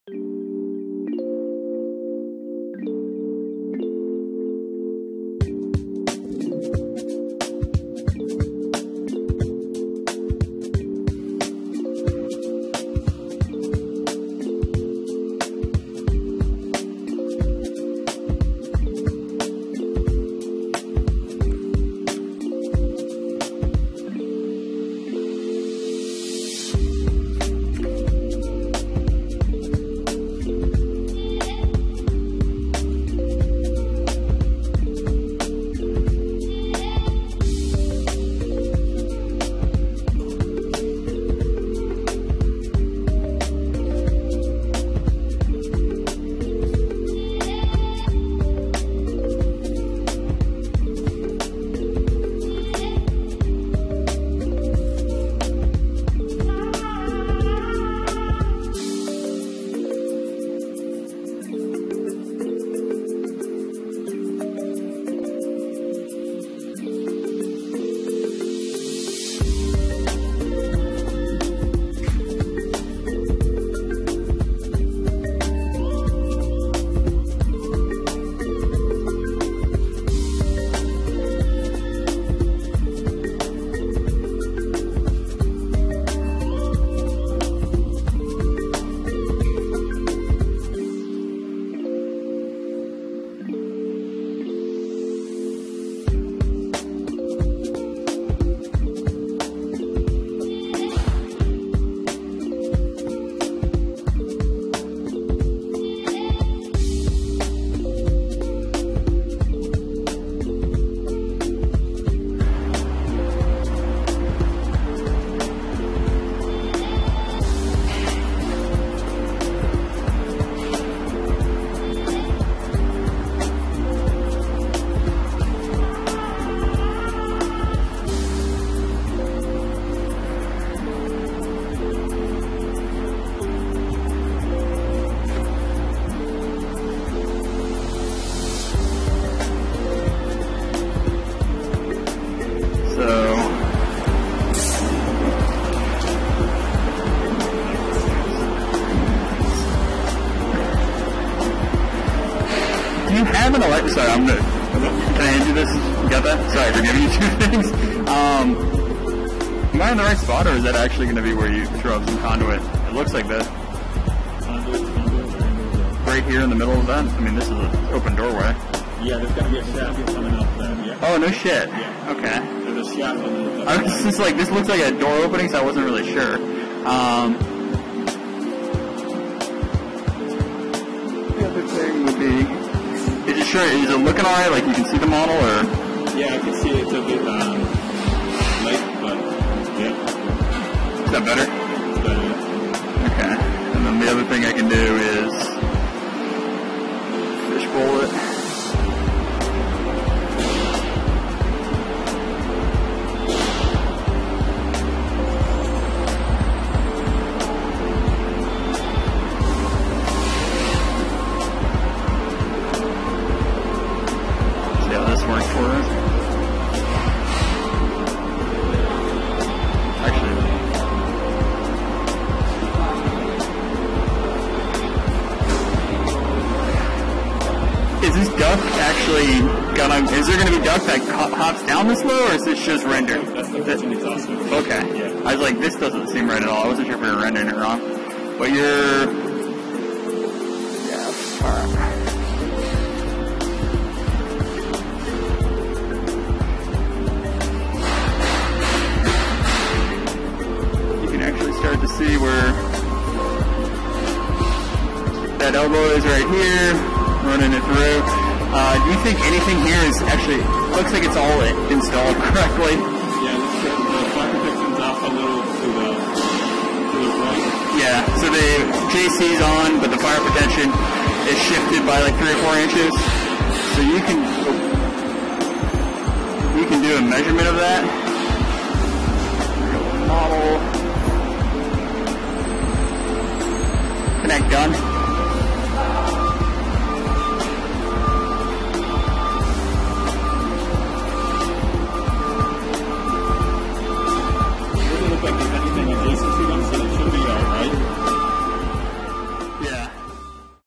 XR10 with HoloLens 2 onsite for QA/QC
XR10 with HoloLens 2 onsite for QA/QC. Align your model onsite to find any potential problems. See the Fire Prevention is a few inches off but shouldn't be a problem.

Load multiple model types from all the subs (DWG, NWD, RVT, IFC, SKP, PDF, etc) into Trimble Connect and then view them onsite with FieldLInk, TrimbleXR10, mobile phone or Tablet.

Compare precisly and accurately with the X7 scanner and fieldlink or share via MicrosoftTeams RemoteAssist live video with the HoloLens2